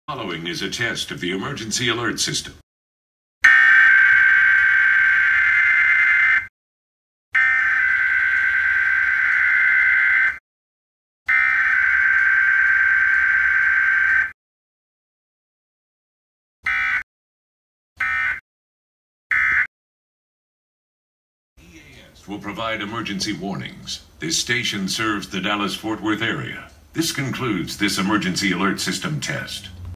WFAA Required Weekly Test (EAS #420) (FIRST VIDEO On iMovie).
I Borrowed Audio From 411 (Same Thing) Because The Volume Is Very Low On This One.